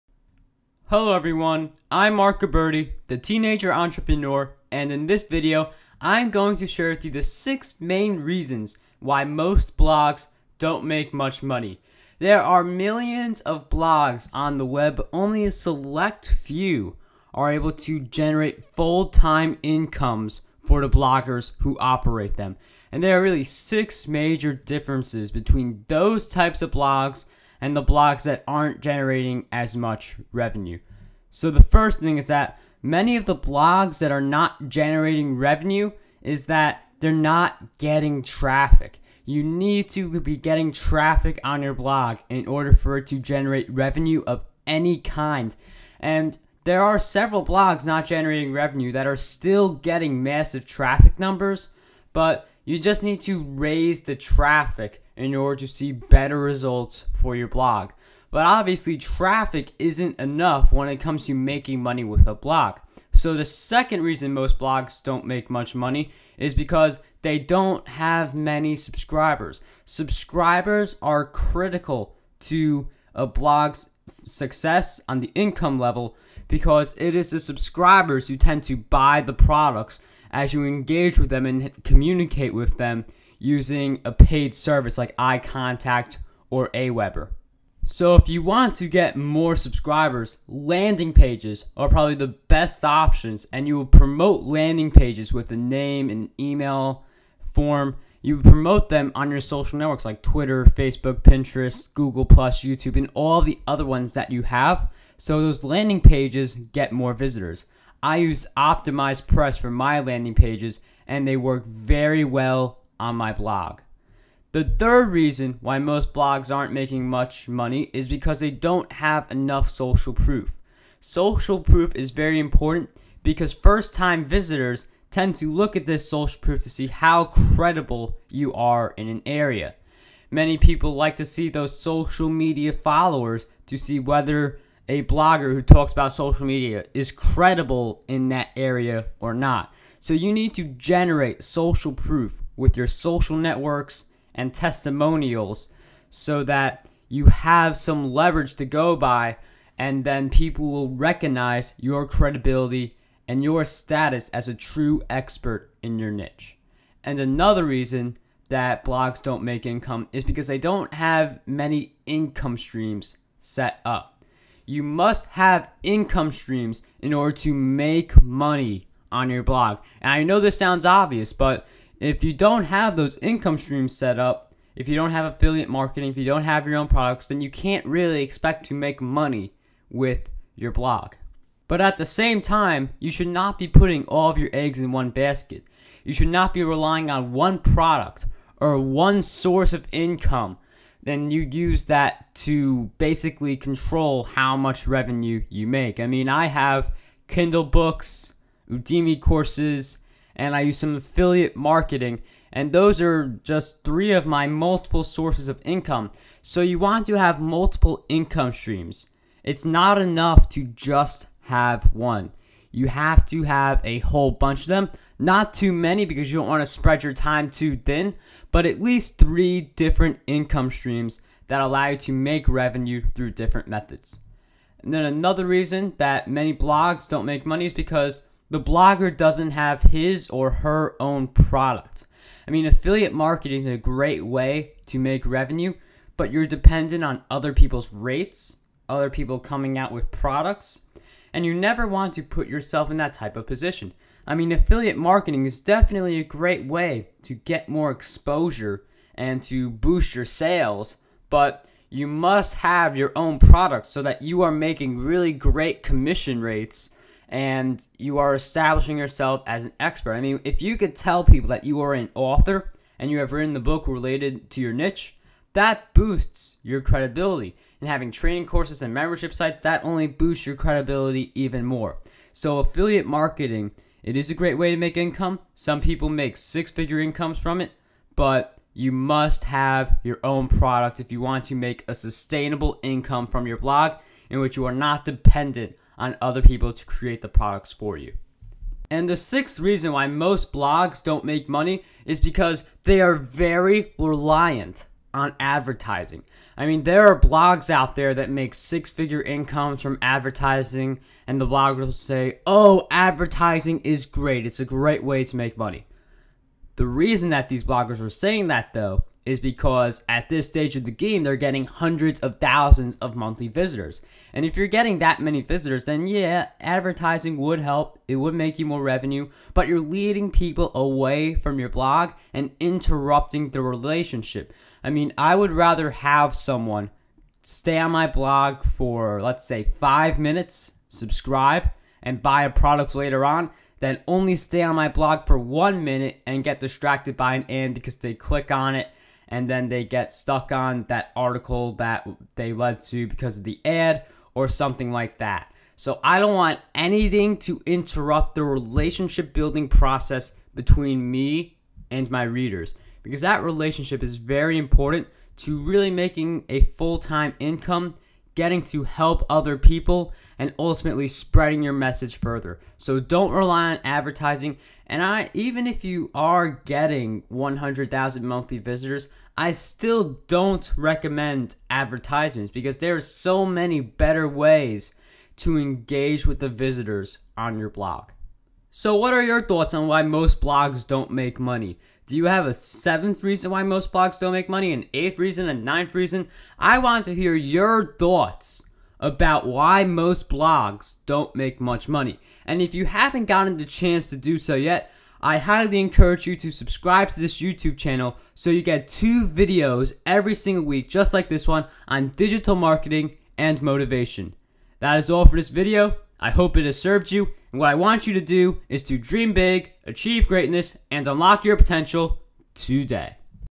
The Six Main Reasons Why Most Blogs Don't Make Money
Millions of blogs get updated on the web every day, but not all of these blogs make revenue. In fact, only a small number of bloggers generate full-time incomes from their blogs. To generate revenue with your blog, you must first learn what is holding your blog back from generating revenue. In this video, I will discuss the six main reasons why most blogs don't make money.